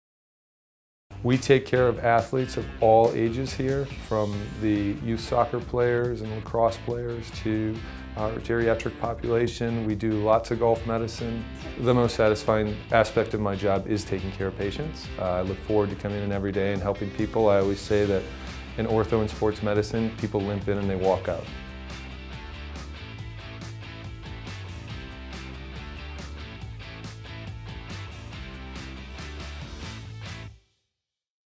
Sports Medicine with Dr. McClone
Casey McClone, MD offers St. Augustine area patients non-surgical treatments to their sports-related injuries. Learn more about Dr. McClone and Orthopaedic Associates of St. Augustine's Sports Medicine Center

Sports medicine specializes in preventing, diagnosing and treating injuries related to participating in sports and or exercise, specifically the rotation or deformation of joints or muscles caused by engaging in such physical activities. Most commonly associated with injuries sustained by athletes, sports medicine also extends to the active adult who participates in sports such as golf, tennis, running, swimming, surfing and other activities.